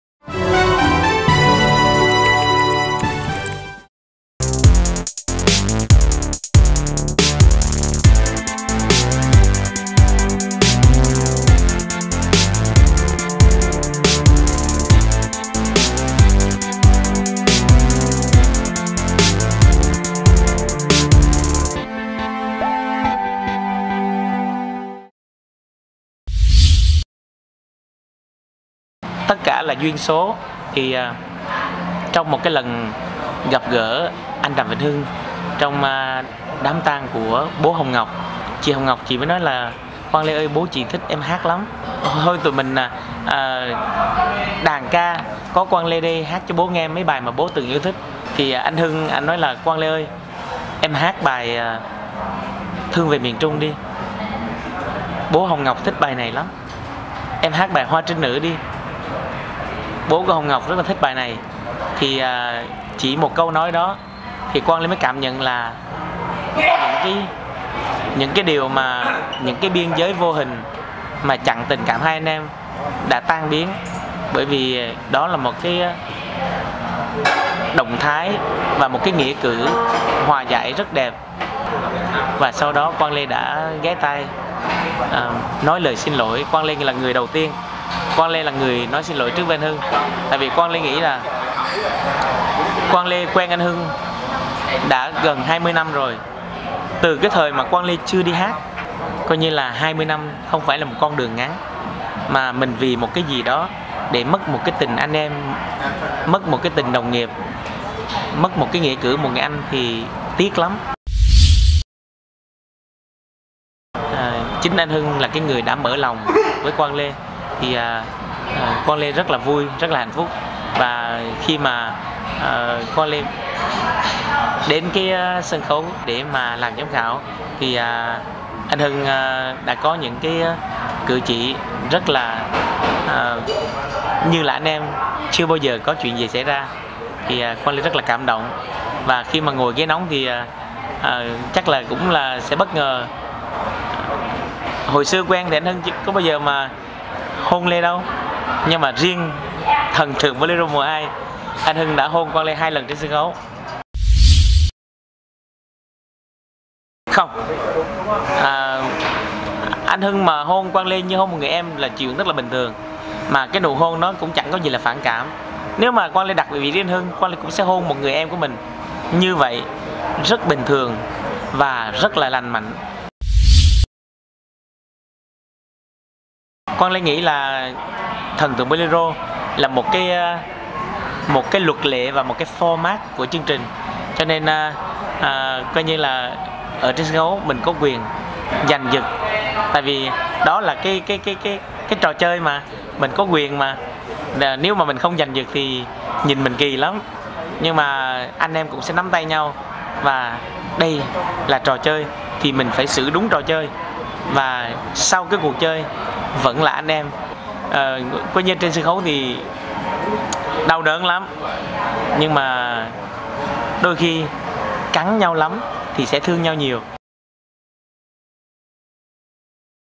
Clip độc quyền ca sĩ Quang Lê nói về nụ hôn của Đàm Vĩnh Hưng
Kênh YouTube chính thức của Báo Thanh Niên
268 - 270 Nguyễn Đình Chiểu, Phường 6, Q.3, TP HCM